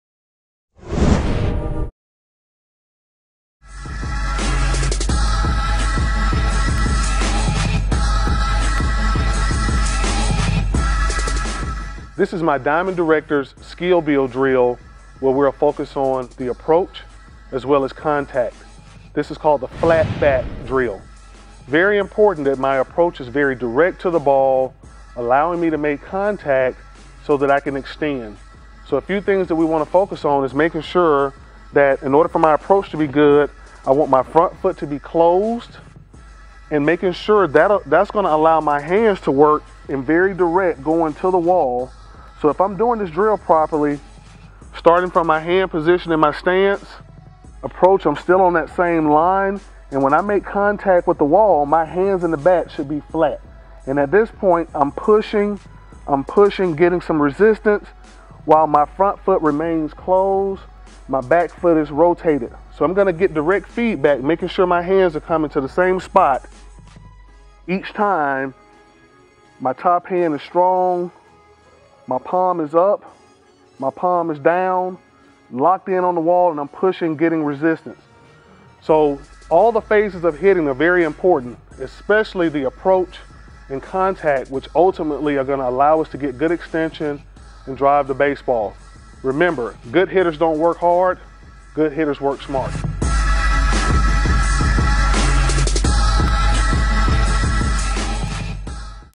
Diamond Directors Skill Build Drill: Wall Drill Flat Bat
The purpose of this drill is establish the proper body alignment at the point of contact.
The hitter will begin this drill from a loaded position with the front foot engaged with the wall and the back foot spread the length of the bat.  Without a stride, direct both hands to the ball maintaining the same hand path from the stance. Simultaneously, the back foot will rotate with the top hand.  Making contact with the wall and the barrel simultaneously, maintain a bend in the arm at contact and hold the position for 5 seconds.  The head should be aligned with the back knee and the ground.